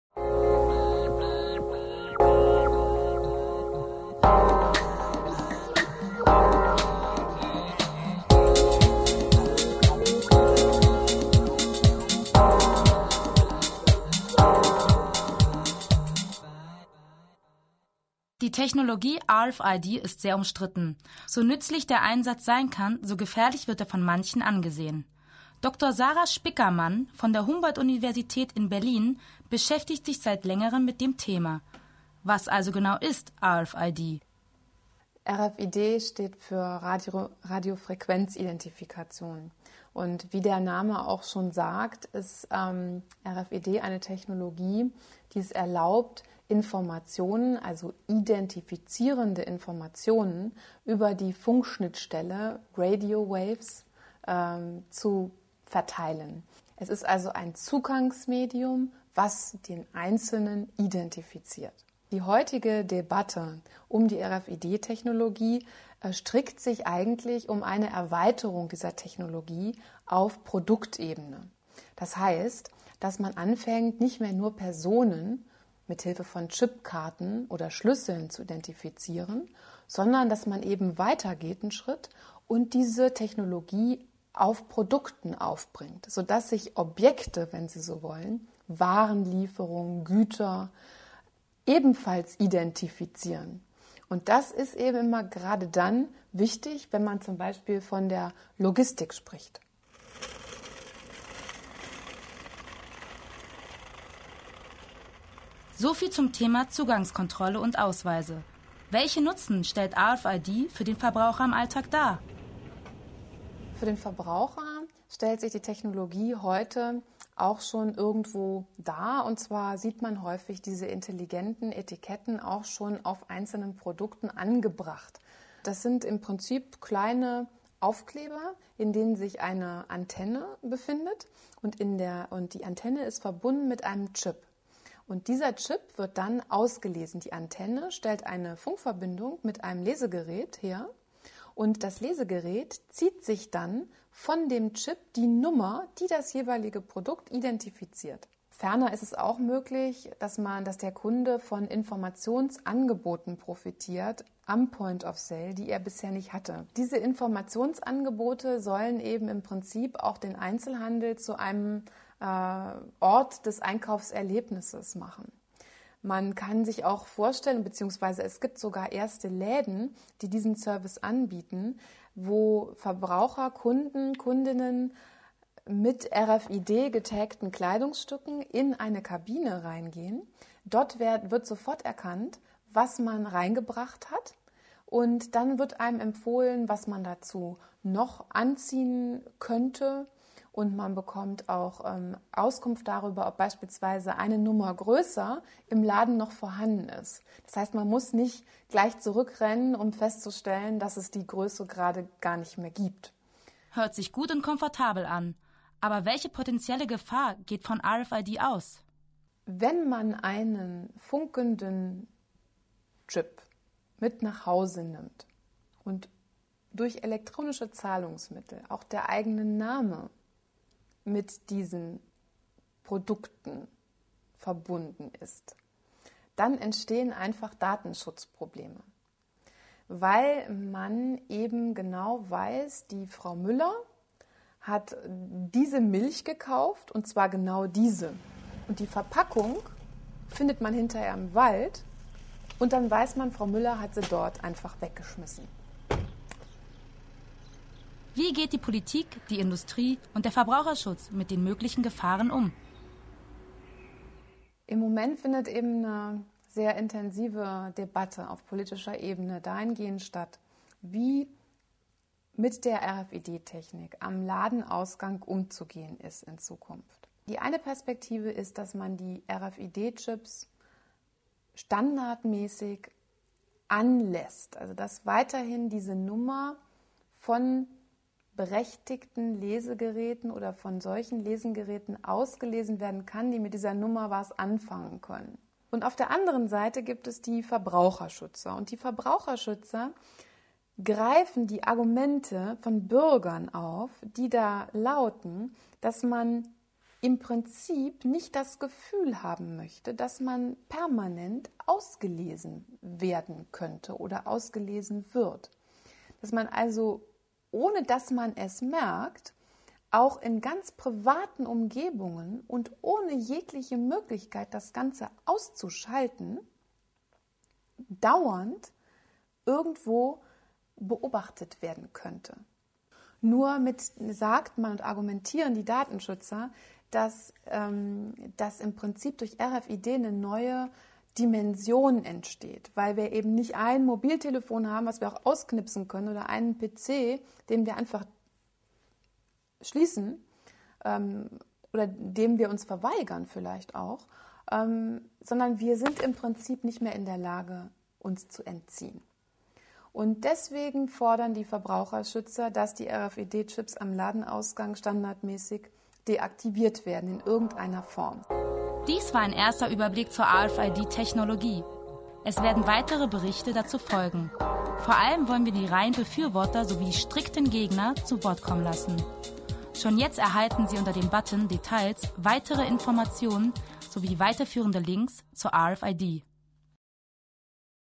RFID - geniale Möglichkeiten und mögliche Gefahren
Von genialen Möglichkeiten und möglichen Gefahren.
Einführung und Überblick zu RFID - neutral betrachtet!